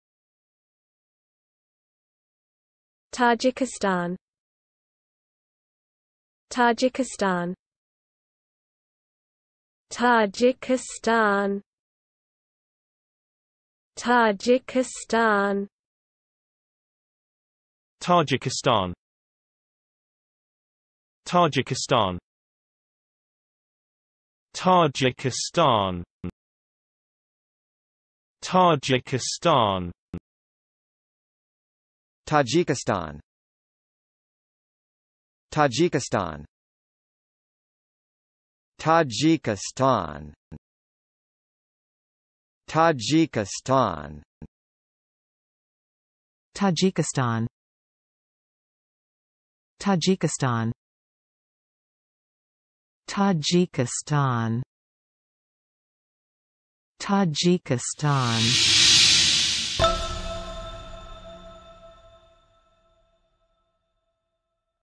How to Pronounce "Tajikistan"
Need help studying English?

Thanks for viewing this video on how to correctly pronounce "Tajikistan".

Oops! We're missing a definition for "Tajikistan", maybe you can help us? Leave a note about Tajikistan in the video comments.

Do you want to request your own word to pronounce.tv?